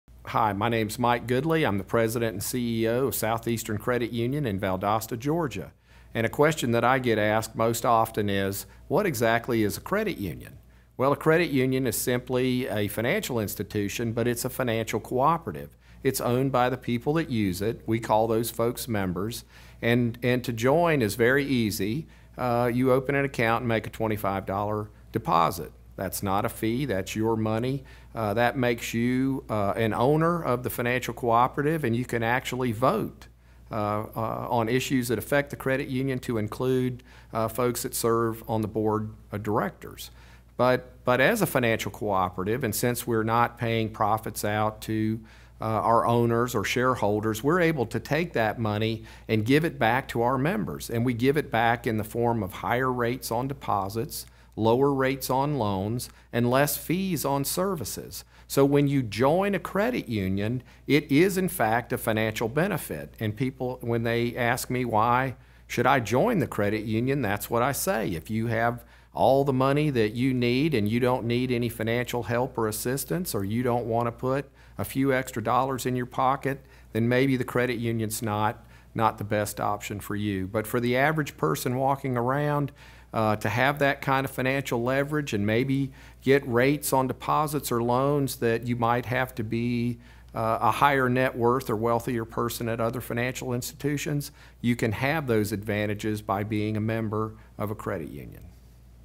What is a Credit Union?
Southeastern Credit Union President/CEO, Mike Gudely, talks about what a credit union is, how they differ from banks, and the advantages of banking with a credit union over a traditional bank; lower rates on loans, higher rates on deposits, fewer fees.

Credit unions are not-for-profit financial institutions that exist to serve their members.